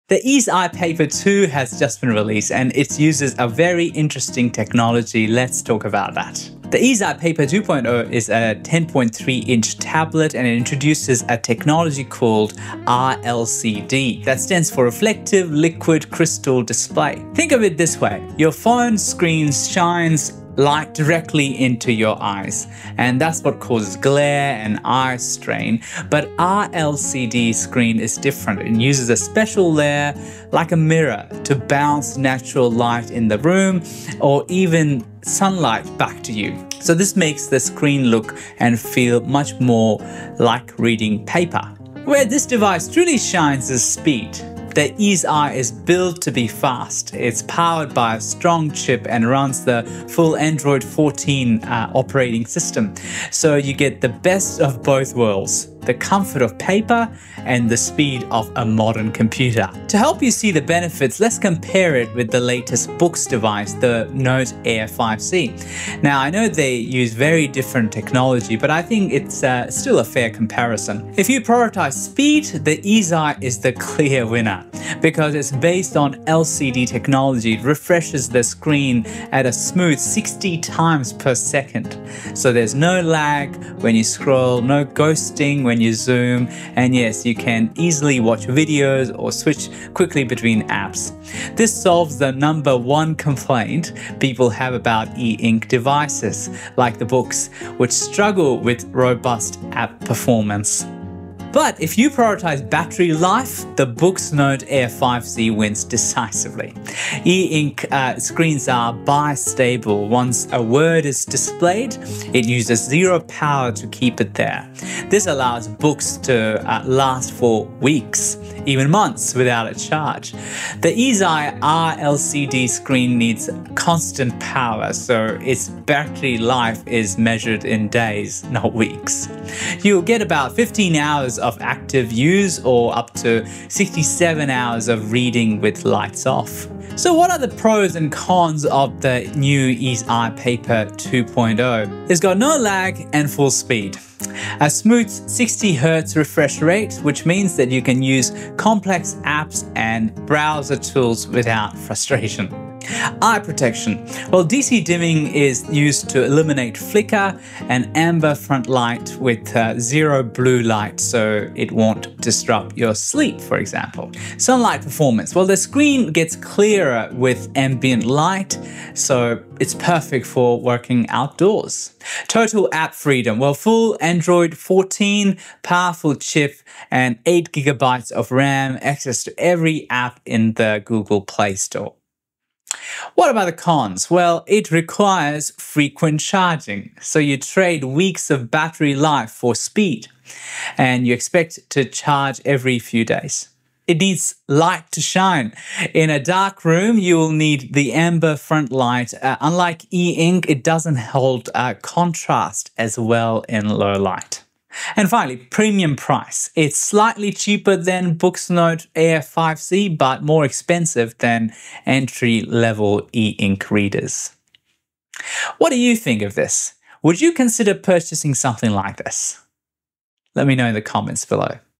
Eazeye Paper 2.0: Is this Alternative to E Ink?
In this commentary video, I talk about the Eazeye Paper 2.0 . It's using a completely new technology called RLCD (Reflective LCD) that aims to solve the biggest problem with E Ink devices: LAG. It boasts a smooth 60Hz refresh rate, full Android 14, and an eye-friendly screen that gets clearer in the sun! But is the trade-off worth it?